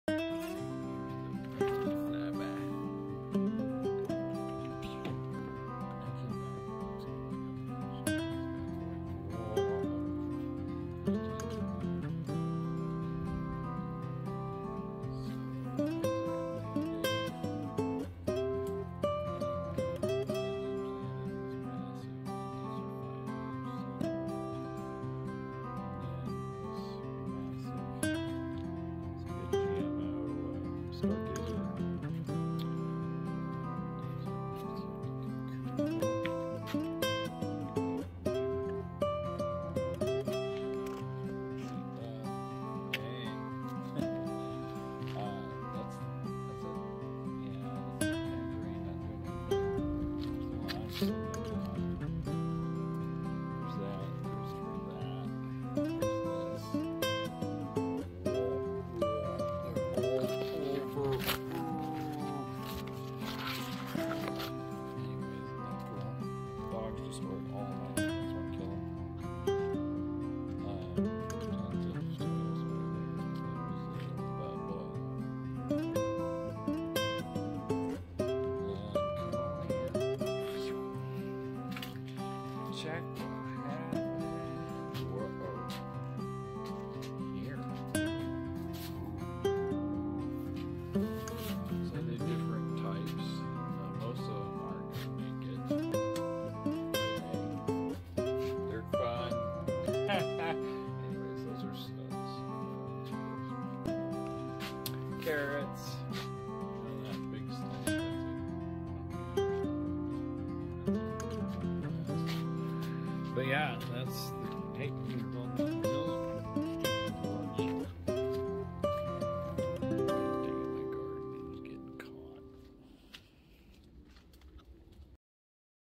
Thank You for Watering💧
Summer blooms are fun as heck, along with our first try at watermelons!